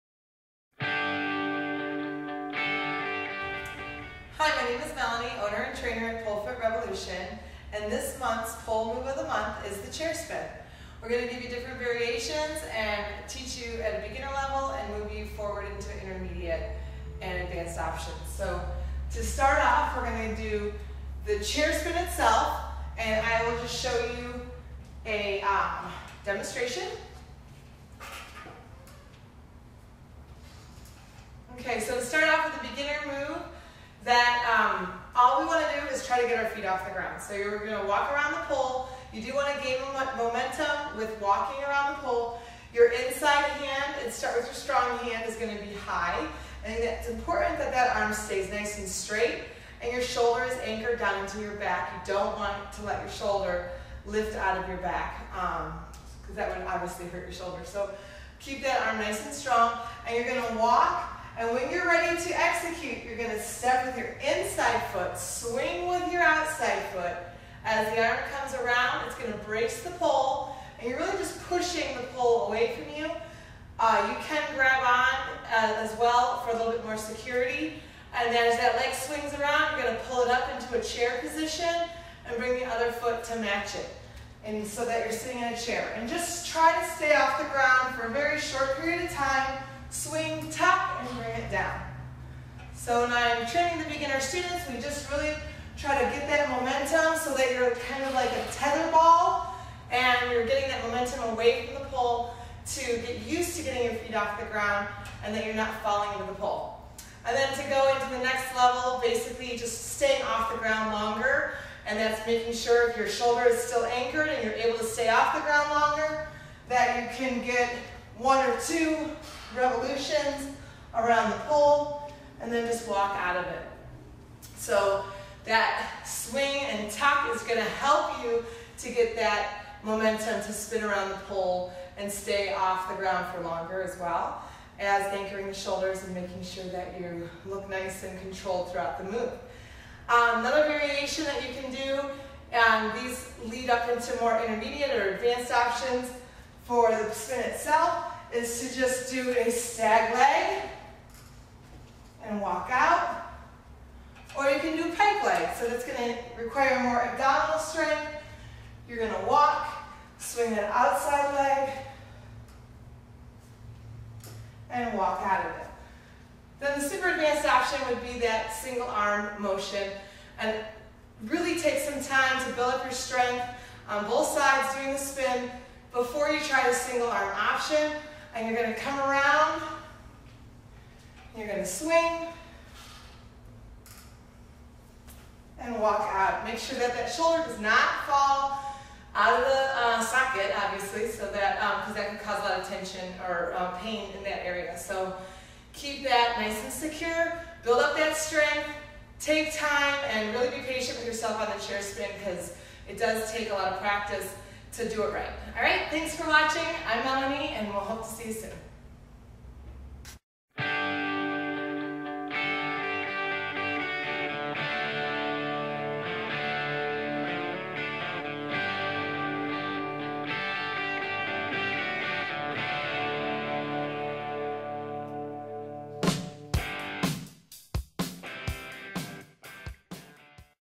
Chair Spin Tutorial
The Chair Spin is a beautiful move that can be used in many ways with combos and transitions.  There are also variations of it for all levels that we teach in this video.  Work to your level and as you gain strength and skill, try the next one. Enjoy!